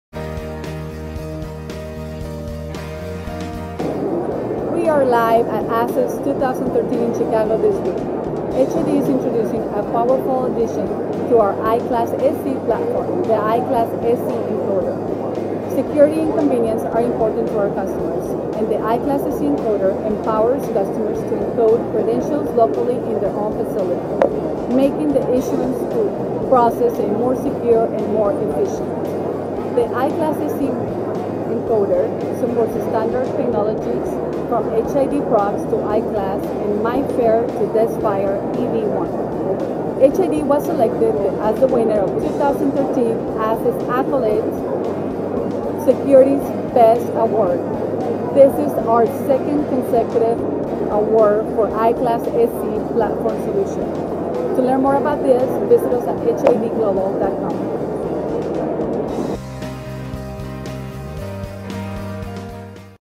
iCLASS SE® Encoder Demonstration
Part of HID Global's iCLASS SE platform, the iCLASS SE Encoder makes it easy to upgrade access control card technologies by allowing users to extract existing data from a credential and write the data to a new one.